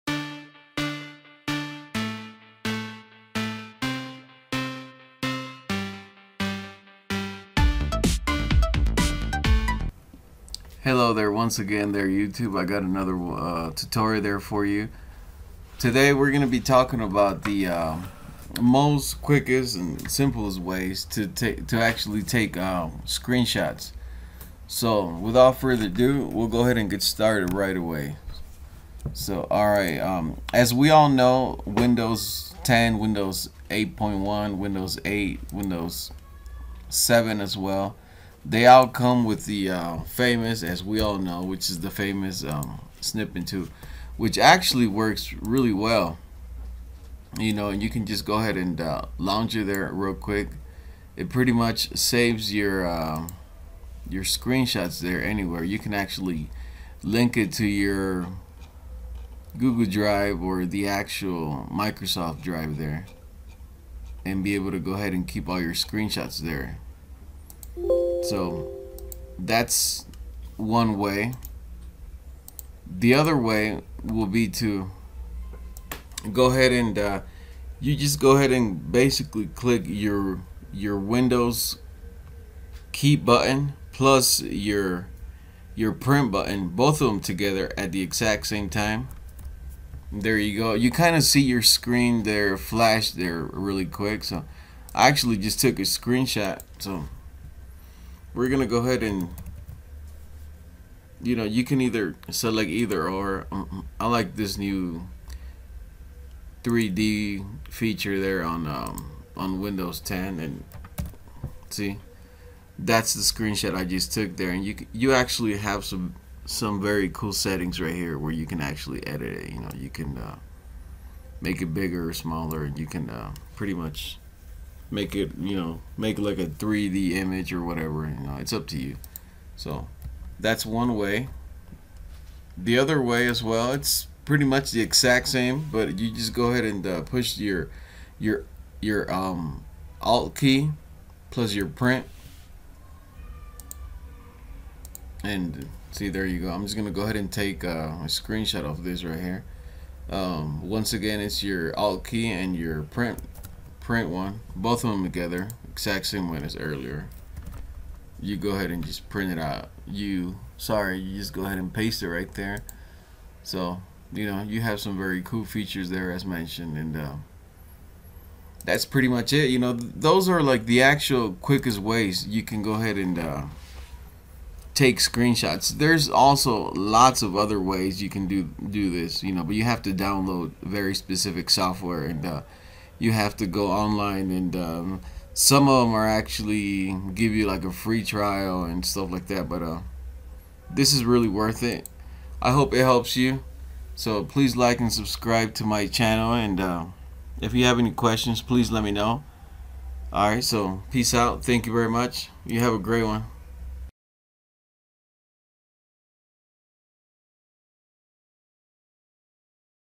3 simple Ways to take Screenshots in Windows 10 /8.1/ 8 /7 Extremely Easy Way
These 3 ways are Probably the quickest ways you can take screenshots in Windows 10, these 3 same ways will also work on Windows 8.1 / 8 / and 7 as well.

No downloads needed at all.